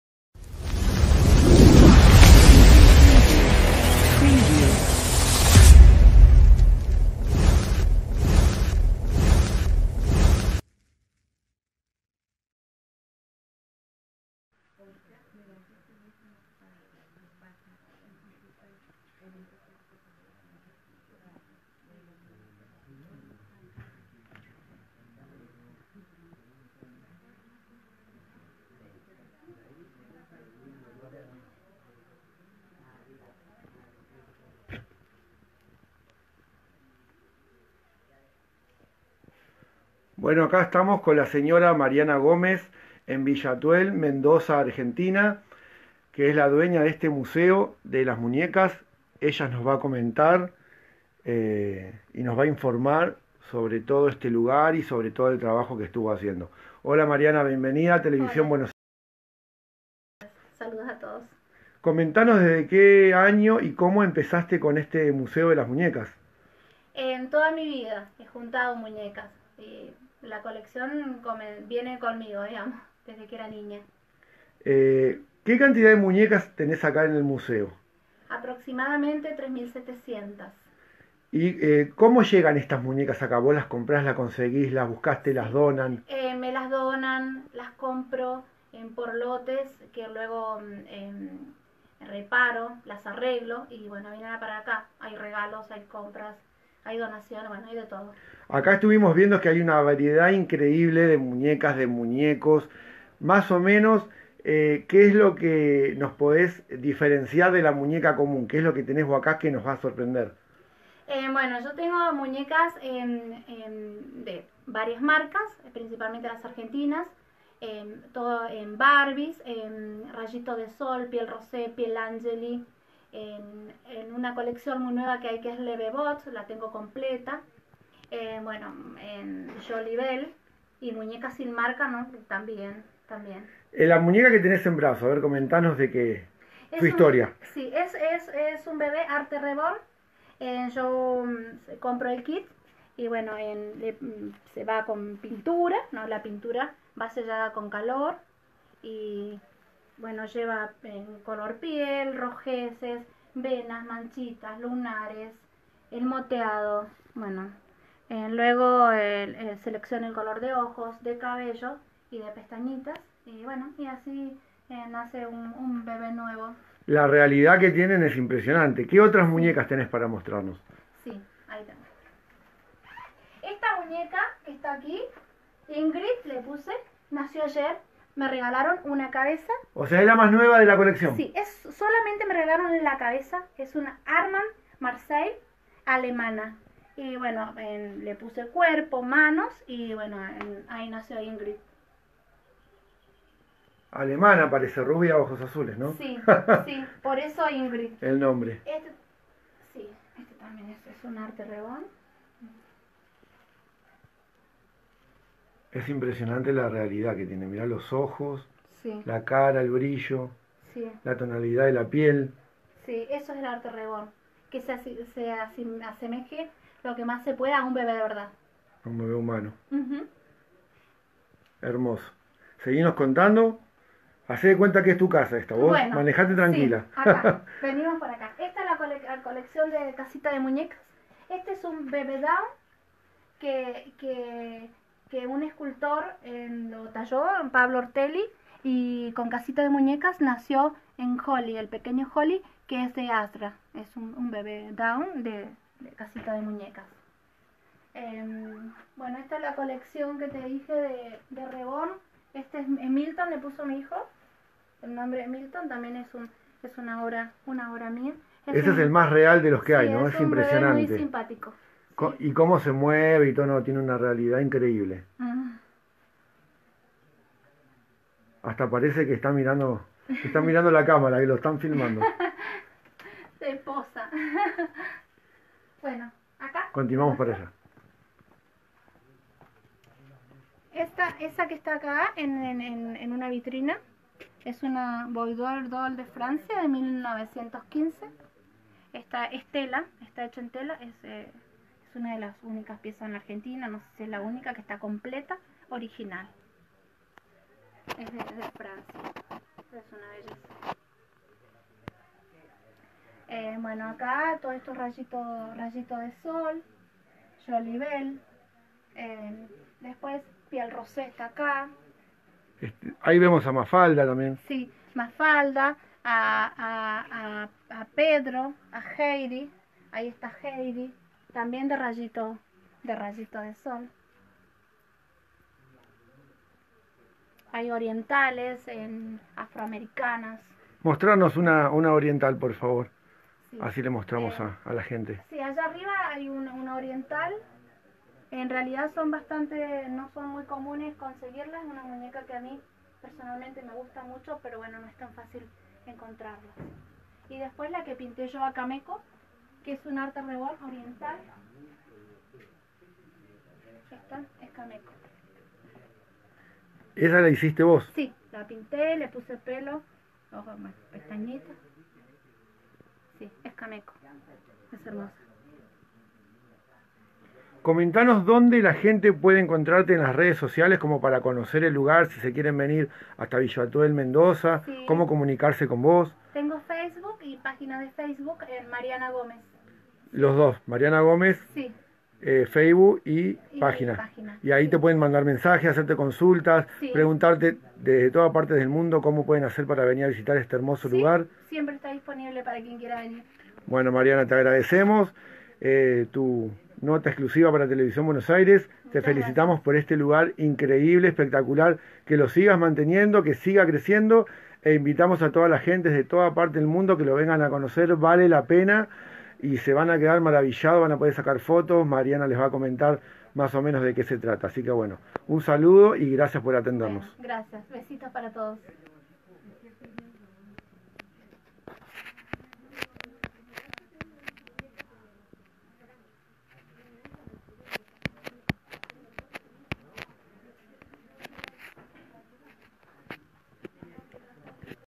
Casa de Muñecas Entrevista a MARIANA GOMEZ en TELEVISIÓN BUENOS AIRES
Casa de Muñecas Entrevista a MARIANA GOMEZ en Villa Atuel Mendoza